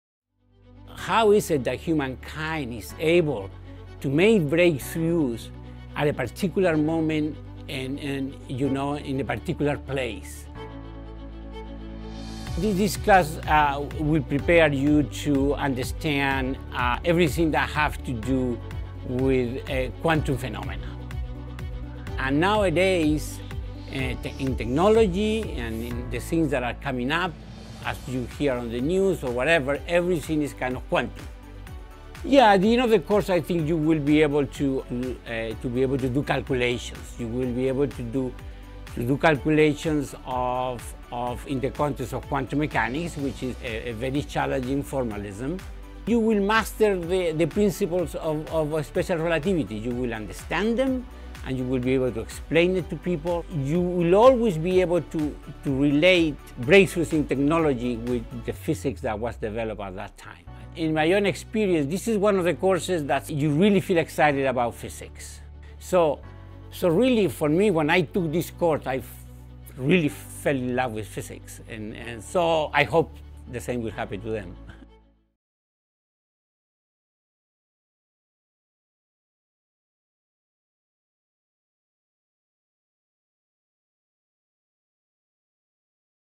PHY 361 - Introductory Modern Physics Course Overview
Arizona State University’s PHY 361 Introductory Modern Physics course explores quantum phenomena, teaches students how to do calculations within the context of quantum mechanics and helps students master the principles of special relativity.

Hear from Ricardo O. Alarcon, a professor of physics and faculty member of Barrett, The Honors College at ASU, as he dives into the important topics covered in this online physics course. He explores how the class deepens students’ knowledge of quantum physics and looks at the extent to which modern physics infiltrates nearly all levels of our world. Alcaron also shares his own personal experiences with and excitement for the course.